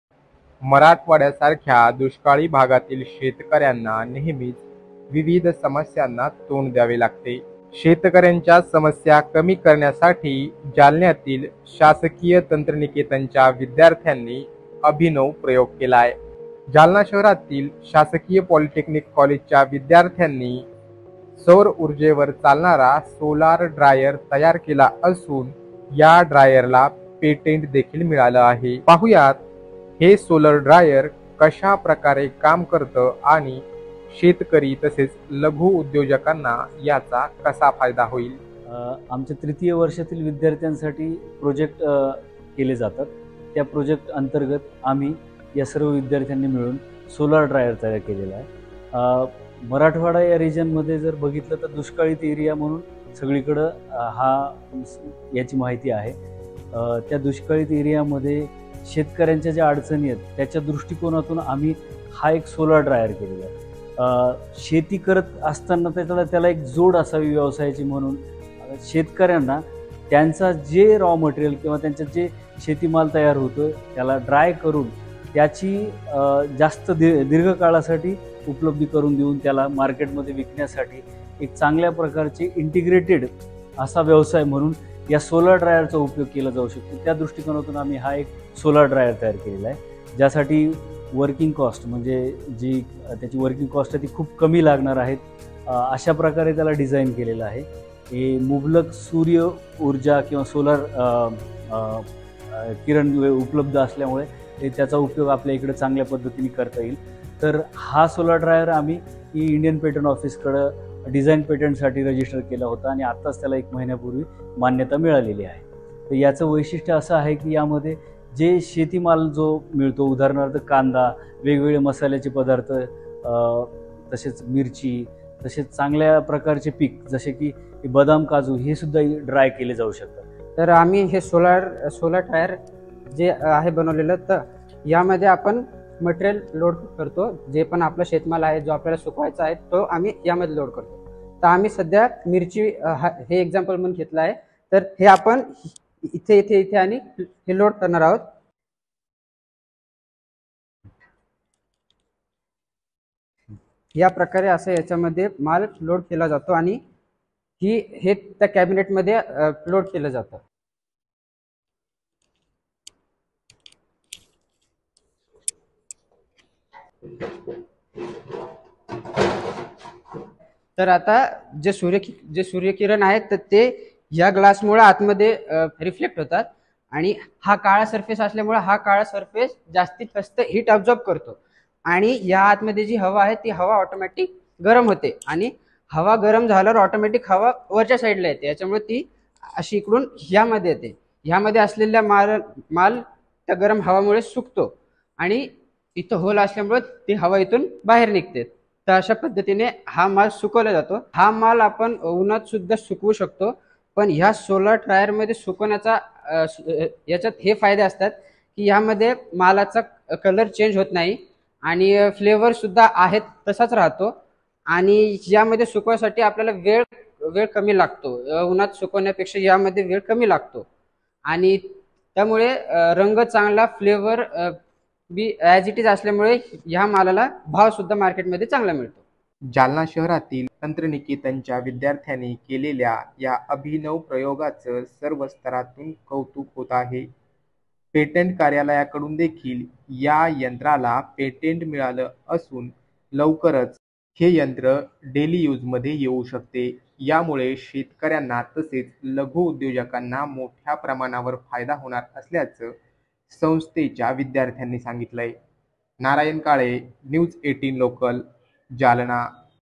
Solar Dryer फक्त साडेतीन हजारांत बनवलं सोलार ड्रायर, शेतकऱ्यांना होणार मोठा फायदा
मराठवाड्यातील शेतकरी विविध समस्यांनी नेहमीचं अडचणीत असतात. त्यांच्या मदतीसाठी जालना शहरातील शासकीय तंत्रनिकेतनच्या विद्यार्थ्यांनी सोलार ड्रायर तयार केले आहे. शेतकऱ्यांबरोबरच लघु उद्योजकांसाठी देखील हे यंत्र अतिशय उपयुक्त ठरणार आहे. शेतमाल अधिक गतीने सुकवून त्याचा रंग, स्वाद आणि गुणवत्ता ही टिकवून ठेवणारे अनोखे सोलार ड्रायर जालन्याच्या शासकीय तंत्रनिकेतनमधील विद्यार्थ्यांनी तयार केले आहे. सौर ऊर्जेवर चालणारे हे उपकरण शेतकऱ्यांसाठी उपयुक्त ठरणारे असून त्याला भारत सरकारच्या डिझाइन पेटंट कार्यालयाने मान्यता दिली आहे.